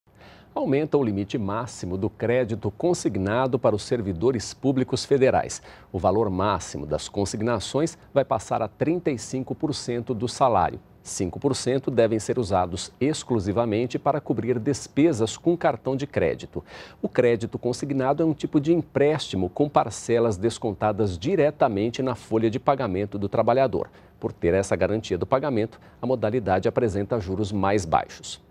Aumenta o limite para crédito consignado de servidores federais
NBR NOTÍCIAS - 14.03.16: Os servidores públicos federais terão mais recursos para uso de crédito consignado. A partir de agora, o valor máximo das consignações será de 35% do salário, sendo 5% usado obrigatoriamente para quitação de cartão de crédito. O crédito consignado é uma modalidade de empréstimo que desconta parcelas diretamente da folha de pagamento, com juros mais baixos.